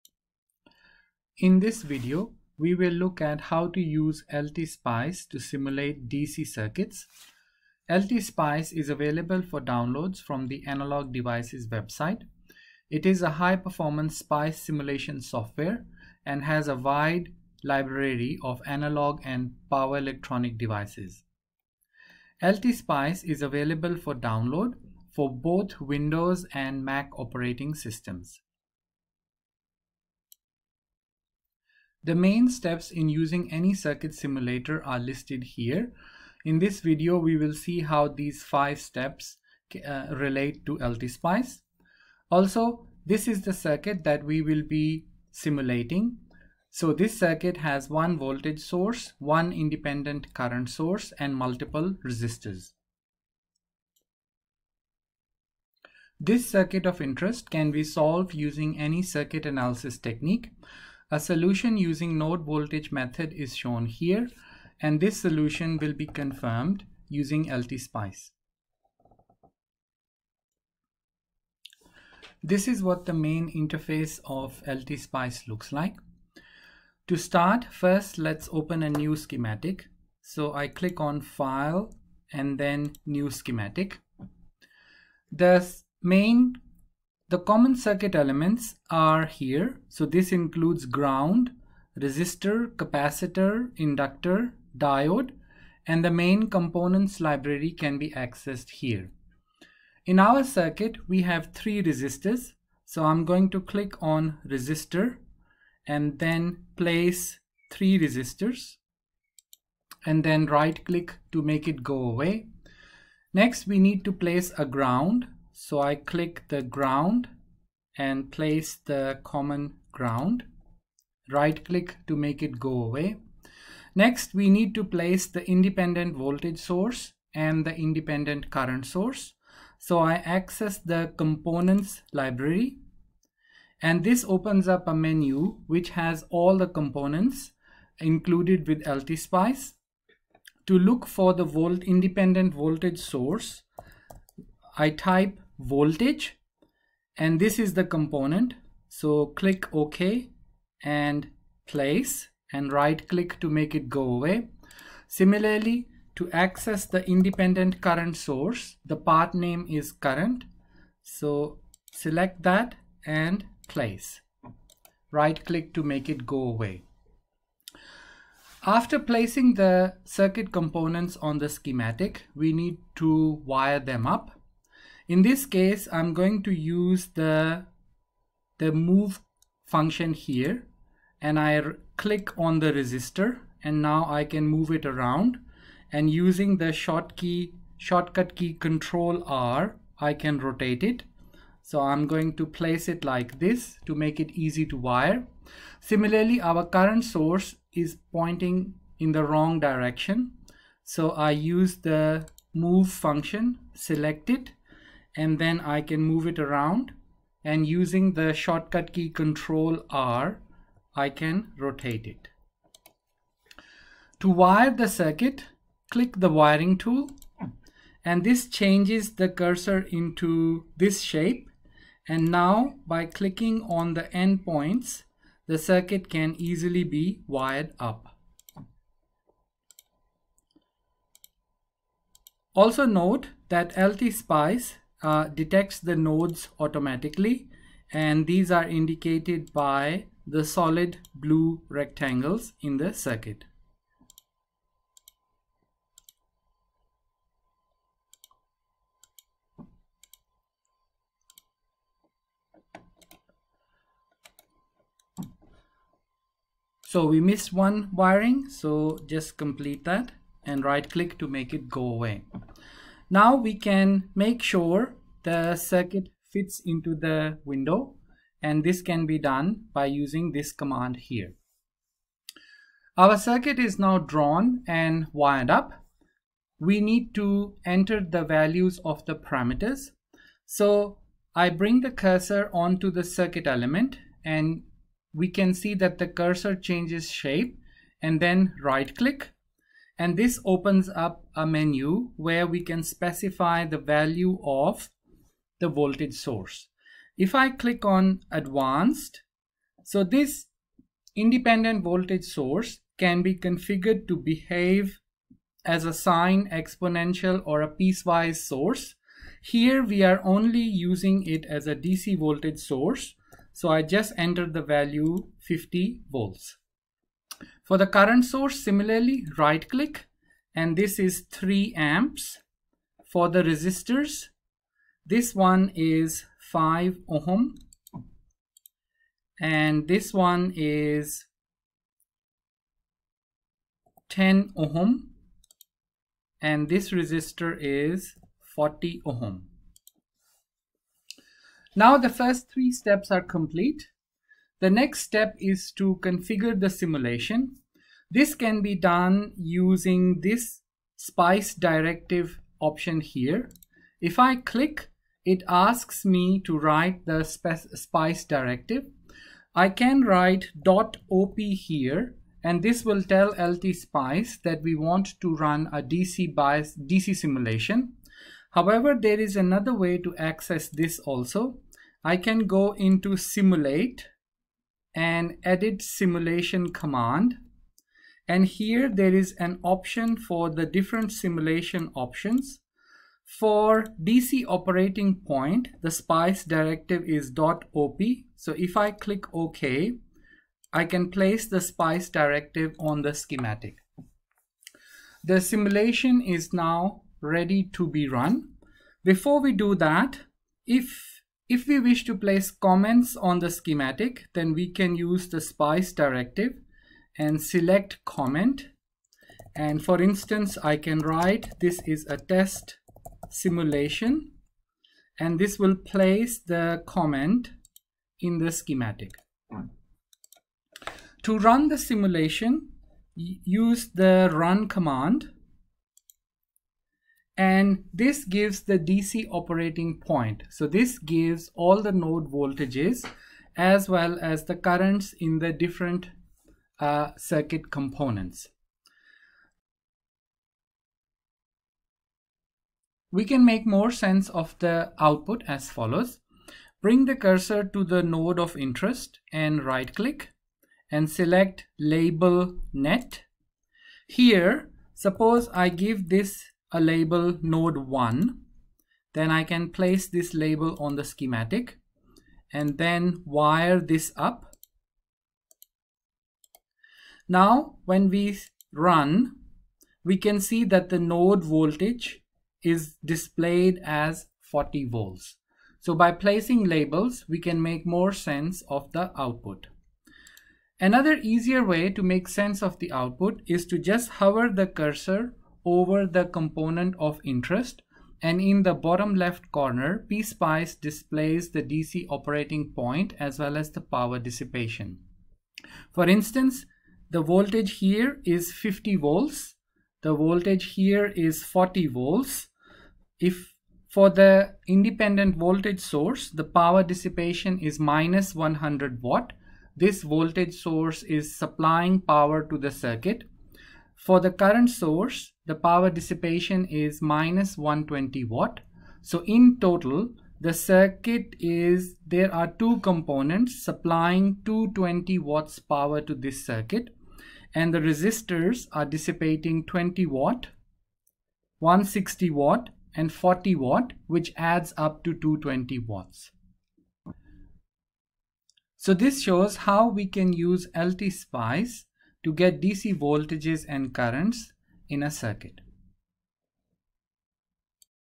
Introduction to LTSPICE for DC Circuit Analysis
This video shows how to use LTSPICE for DC Circuit Analysis and simulate a circuit containing independent voltage and current sources and resistors.